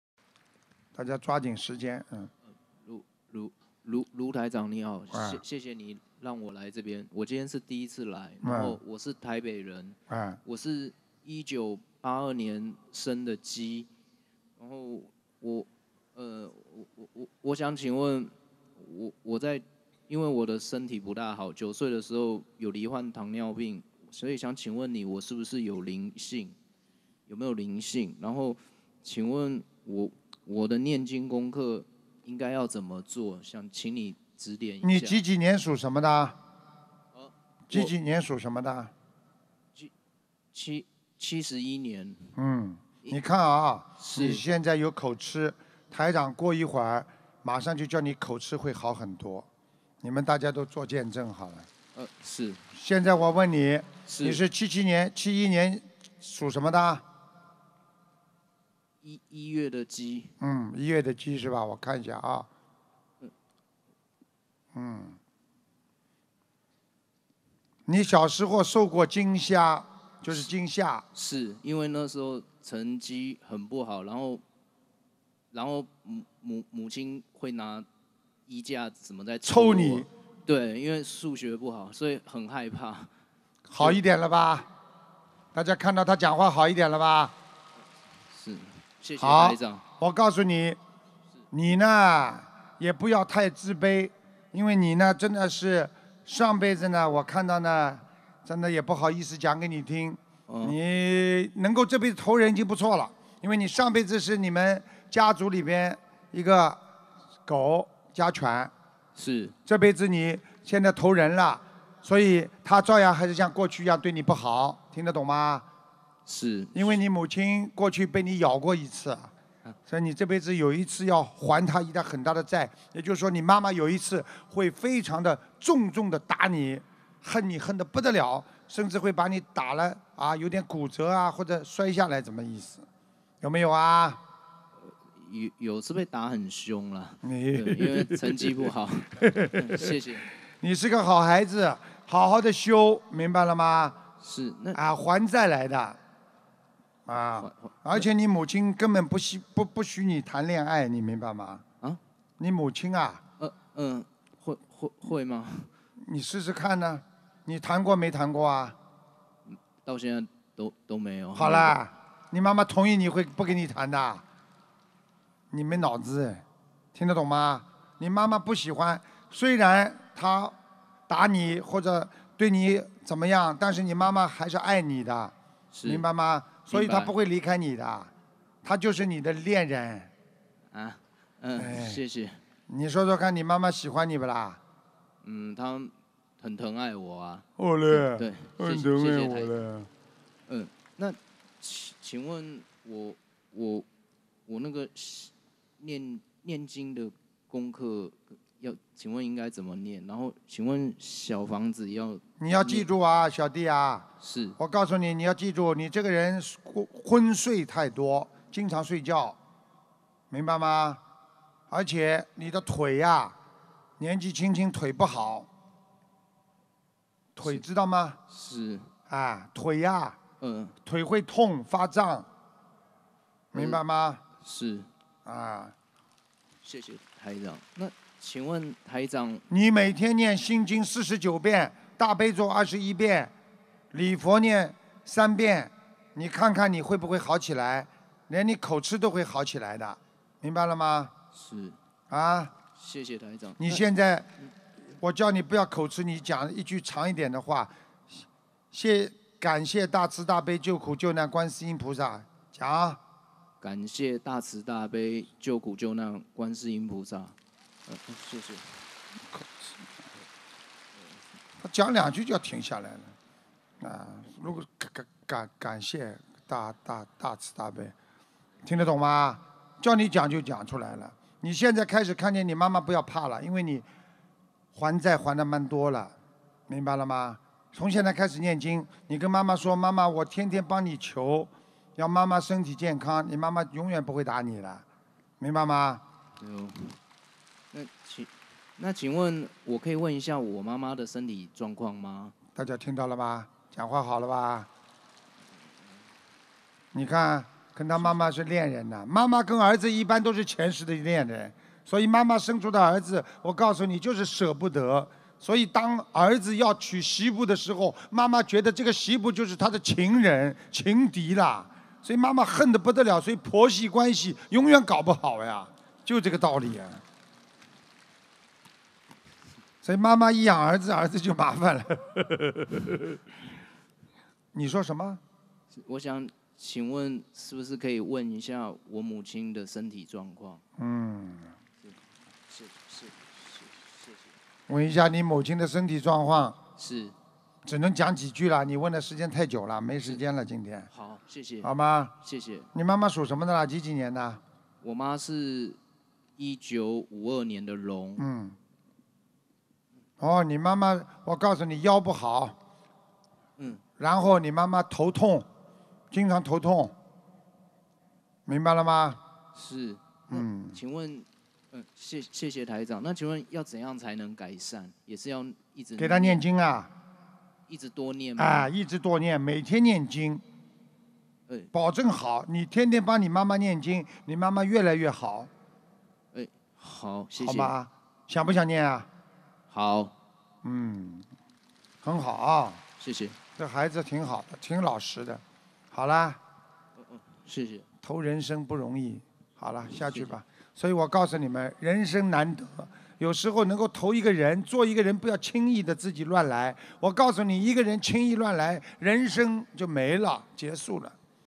【精彩图腾】前世是家族里的狗 台长当场治疗口吃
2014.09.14 | 中国·台北  Taibei, China

心灵法门一切书籍皆免费结缘，🍁加拿大观音堂文化中心欢迎您🍁